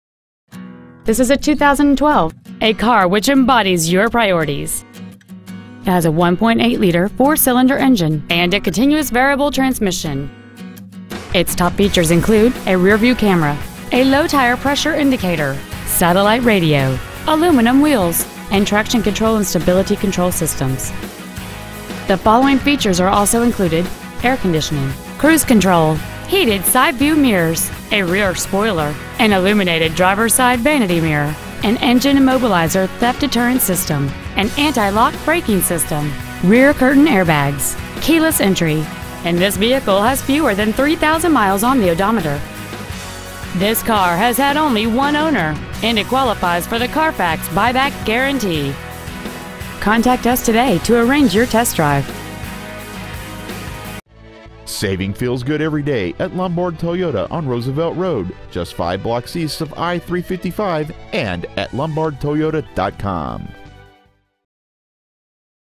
2012 Toyota Prius v Lombard IL 60148
We are proud to present this 2012 Toyota Prius v .

 Year : 2012

 Make : Toyota

 Model : Prius v

 Engine : 1.8L

 Trans . : Variable

 Exterior : Classic Silver Metallic

 Miles : 2,668

 Lombard Toyota

 725 W. Roosevelt Road